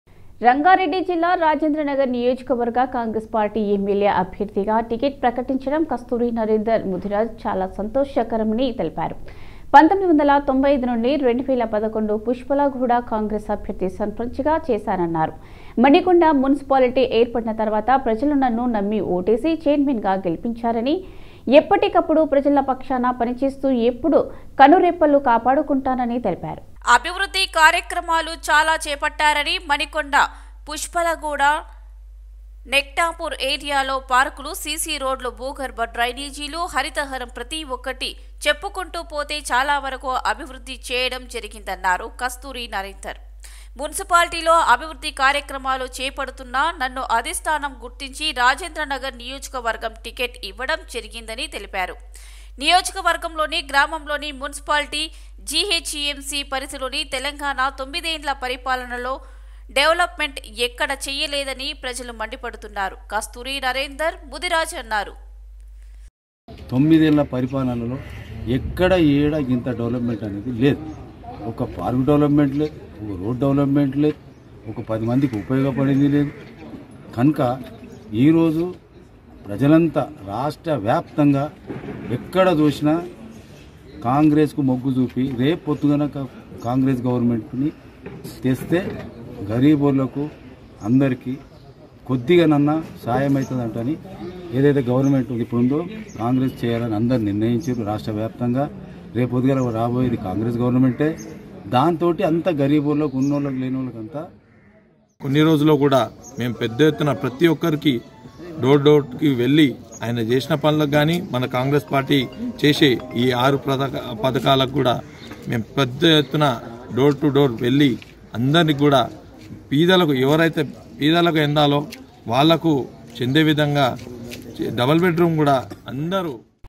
టికెట్ కేటాయింపుపై కస్తూరి నరేందర్ ముదిరాజ్ హర్షం
టికెట్ కేటాయింపుపై కస్తూరి నరేందర్ ముదిరాజ్ హర్షం || @sitivisionlive sitivision @sitivisionlive sitivisionlive @sitivision